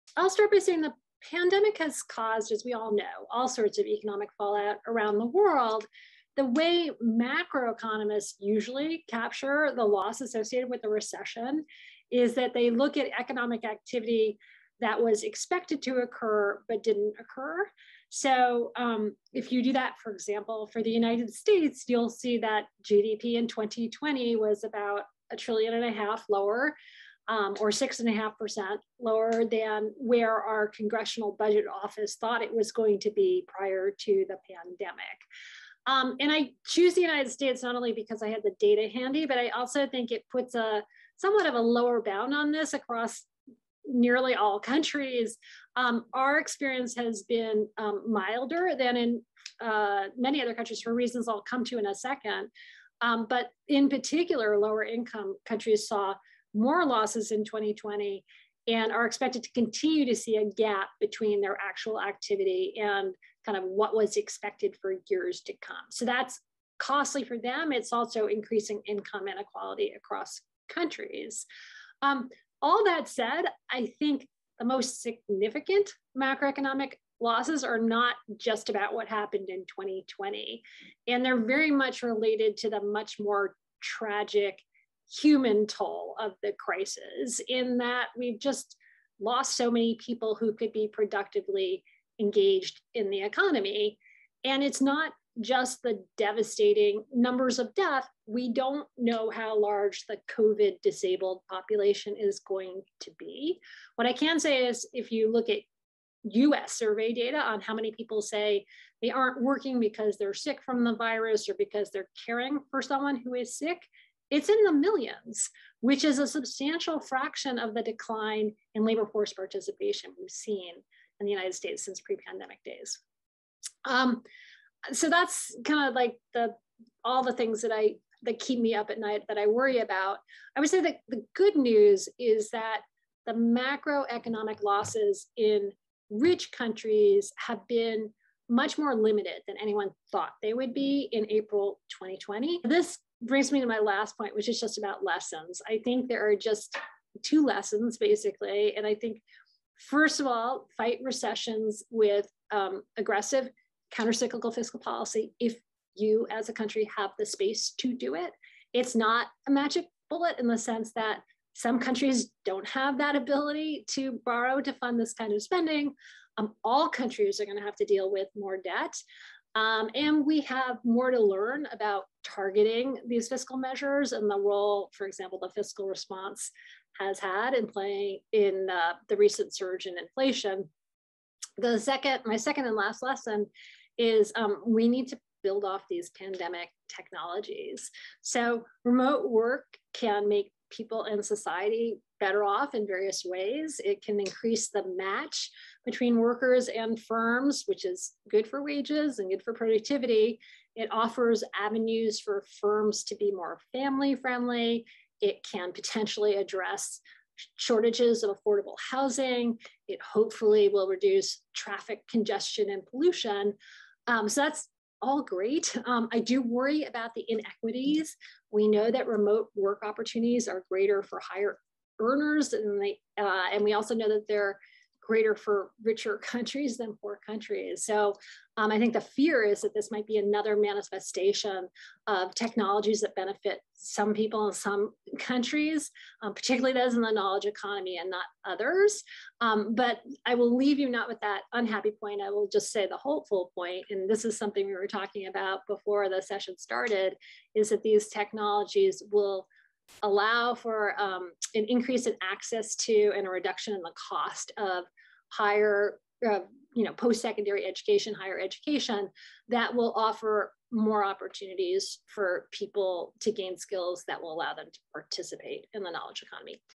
CID Faculty Expert Karen Dynan: How to Promote Inclusive Economic Growth
As part of the Beyond COVID Speaker Series, CID Director Asim I. Khwaja asked Karen Dynan, Professor of the Practice in the Department of Economics at Harvard University, about the biggest macroeconomic loss created by the pandemic, some signs of hope for economies in the response efforts, and most importantly, what lessons can we take forward to overcome losses and promote more inclusive, sustainable growth.

Speaker Bio:
Karen Dynan served as Assistant Secretary for Economic Policy and Chief Economist at the U.S. Department of the Treasury from 2014 to 2017, where she led analysis of economic conditions and development of policies to address the nation’s economic challenges. From 2009 to 2013, Dynan was vice president and co-director of the Economic Studies program at the Brookings Institution.

Before that, she was on the staff of the Federal Reserve Board, leading work in a number of areas, including macroeconomic forecasting, household finances, and the Fed’s response to the financial crisis. Dynan also served as a senior economist at the White House Council of Economic Advisers from 2003 to 2004 and as a visiting assistant professor at Johns Hopkins University in 1998. Dynan’s research focuses on fiscal and other types of macroeconomic policy, consumer behavior, and household finances. Dynan received her Ph.D. in economics from Harvard University and her A.B. from Brown University.